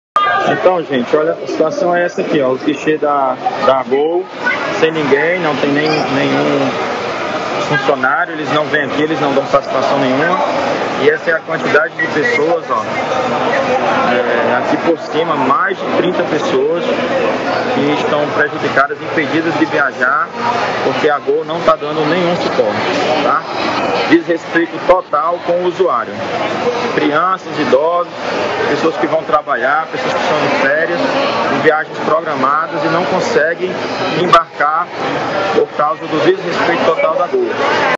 Clientes relatam check-in antecipado e impedimento para embarcar em voo
Imagens mostram fila de passageiros, entre crianças, adultos e idosos, visivelmente irritados com o problema. Vídeo também exibe ausência de atendentes nos balcões da empresa, até para prestar esclarecimentos.